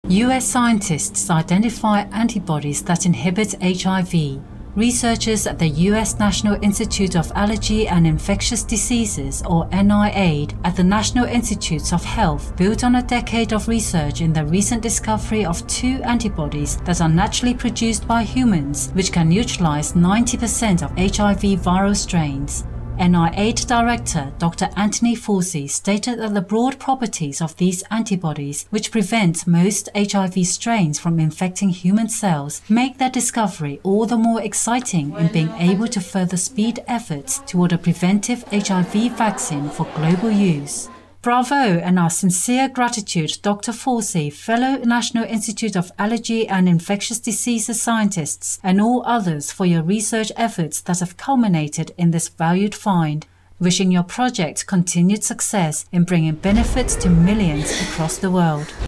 Antibody find spurs fresh hopes for AIDS vaccine
US scientists identify antibodies that inhibit HIV. Researchers at the US National Institute of Allergy and Infectious Diseases (NIAID) at the National Institutes of Health built on a decade of research in their recent discovery of two antibodies that are naturally produced by humans, which can neutralize 90% of HIV viral strains. NIAID Director Dr. Anthony Fauci stated that the broad properties of these antibodies, which prevent most HIV strains from infecting human cells, make their discovery all the more exciting in being able to further speed efforts toward a preventive HIV vaccine for global use.

Bravo and our sincere gratitude, Dr. Fauci, fellow National Institute of Allergy and Infectious Diseases scientists and all others for your research efforts that have culminated in this valued find. Wishing your project continued success in bringing benefit to millions across the world.